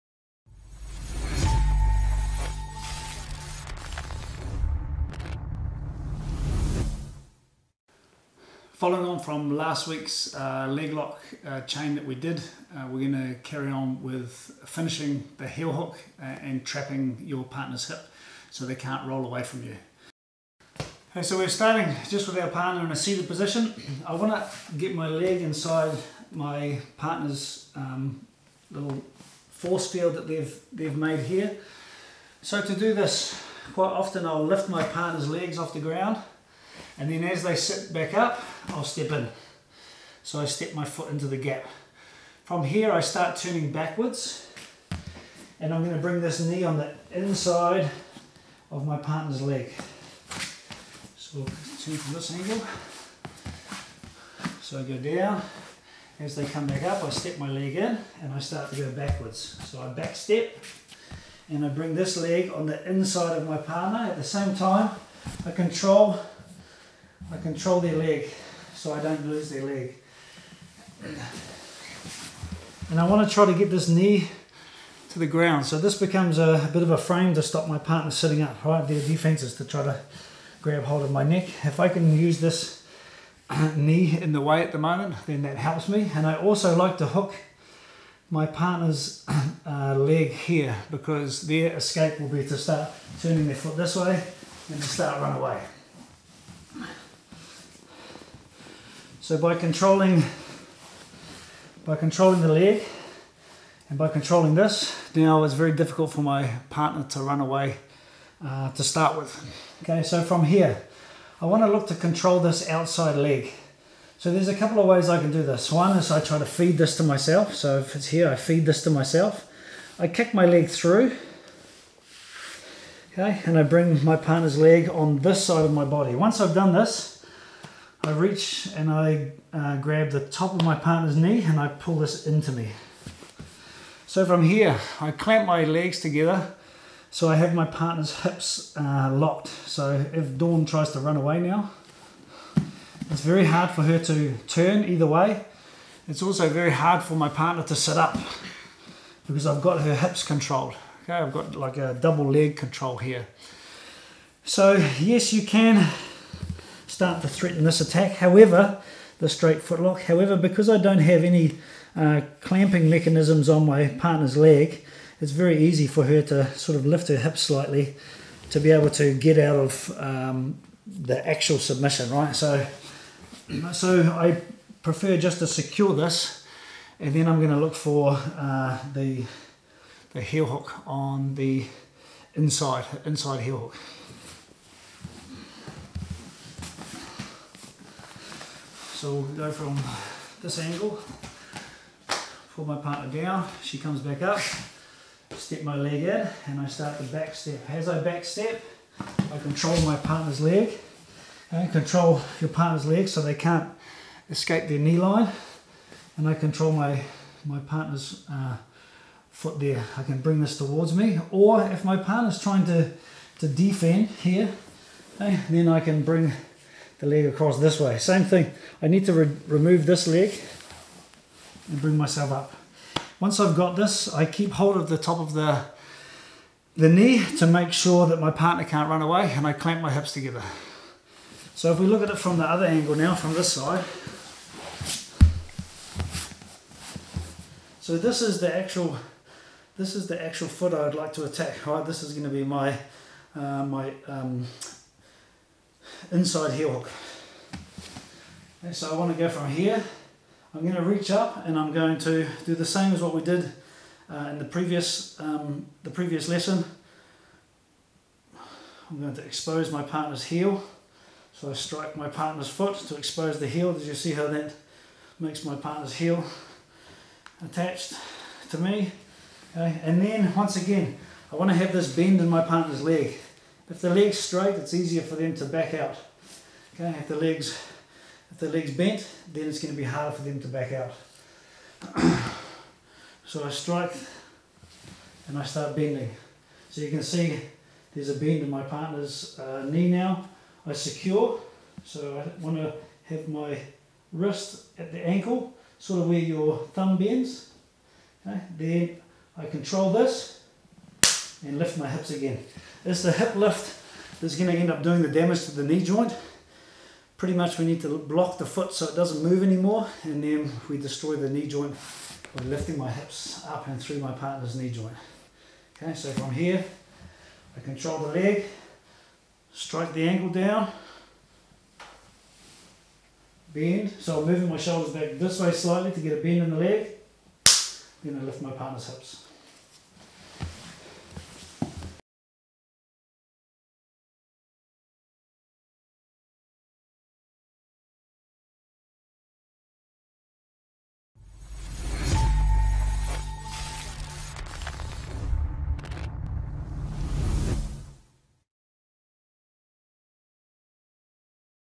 Saddle position to Heel Hook finish
In this video we look at the inside heel hook submission from the saddle. The position itself is known by many names, inside sankaku, 411, the honey hole. These are all essentially the same position, we look at a back step entry, double leg and hip control to the finish.

WARNING: This is an advanced technique that needs to be applied slowly when practicing. you should also note that there is not much movement from the off position to something being damaged. Once you feel some slight tension in the knee or ankle joint... TAP.

At Coast Academy we have a catch and release policy for any heel hook, and these can only be performed by belt belts or higher in a no-gi roll.